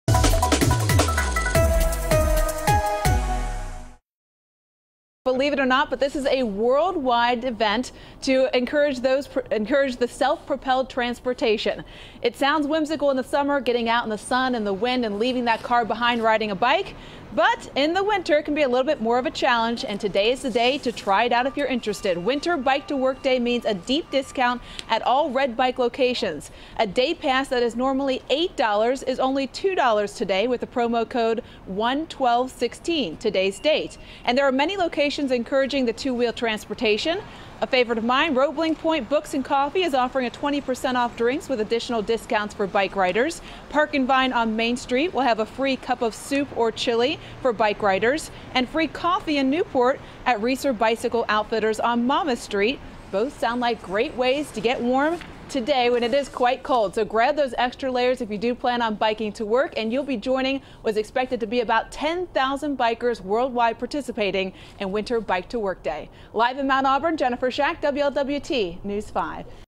Winter Bike to Work Day brings Red Bike discount
Sure, you'll create your own wind chill, but don't let that stop you!